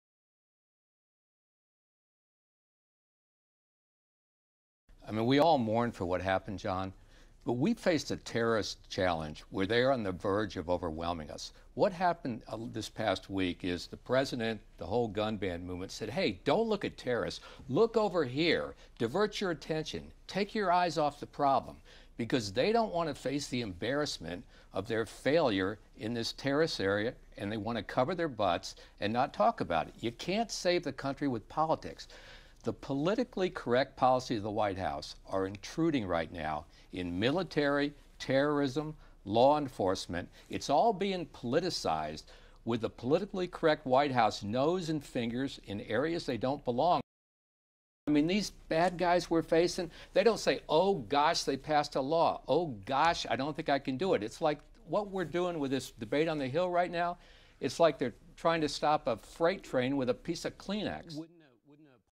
NRA head says Obama politicizing gun debate after Orlando
The head of the NRA (National Rifle Administration)dismissed a renewed effort to tighten controls on access to automatic weapons in the wake of the Orlando massacre on Sunday, saying it was drive by "politics".